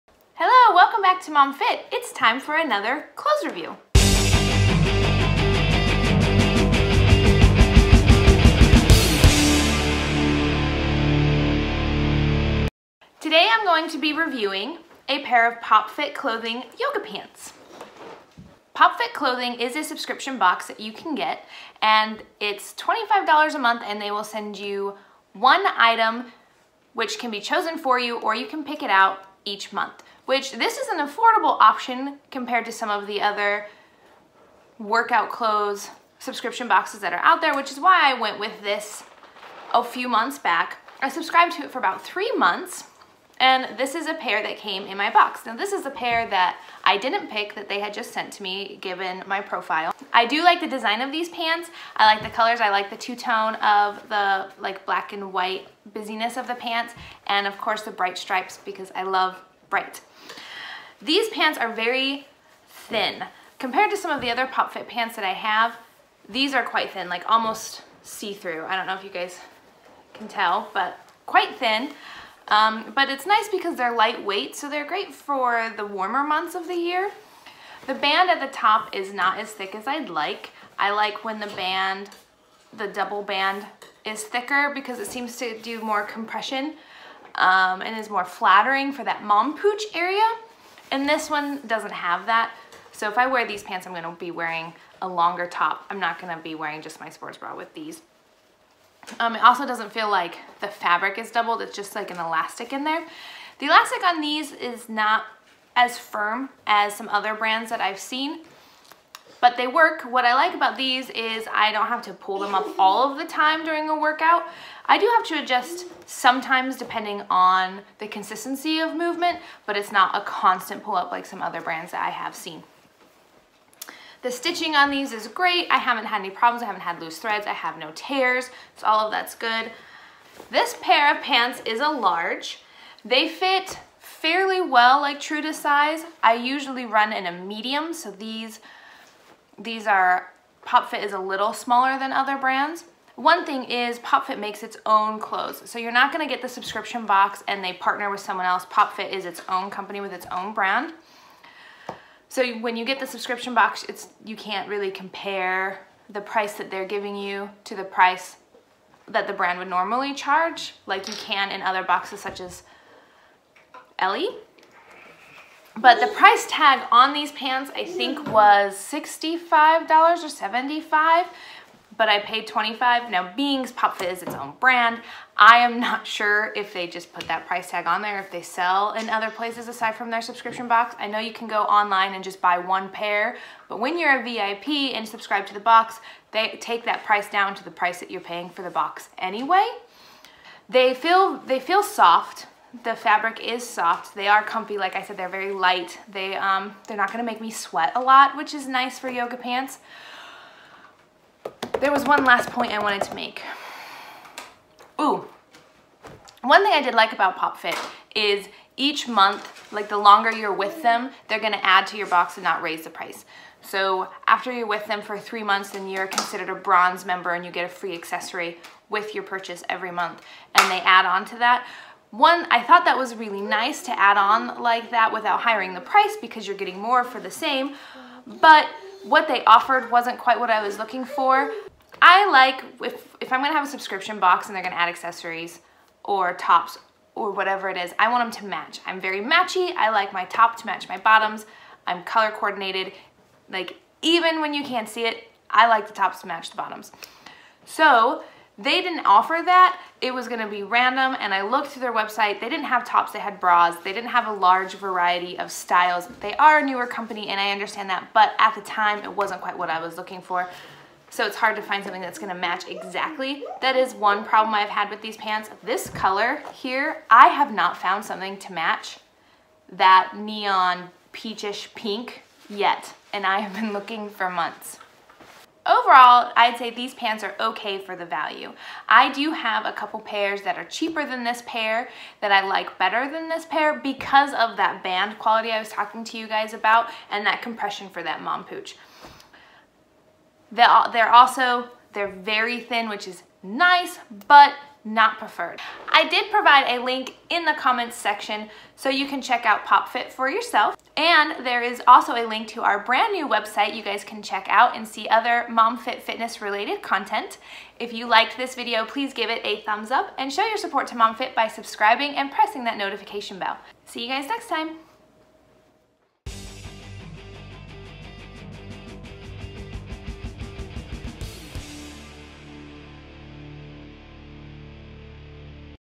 MomFit Review on Pop Fit Clothing Neon Stripe Yoga Pants
Looking to buy some yoga pants?
I review PopFit subscription box outfit here in this video.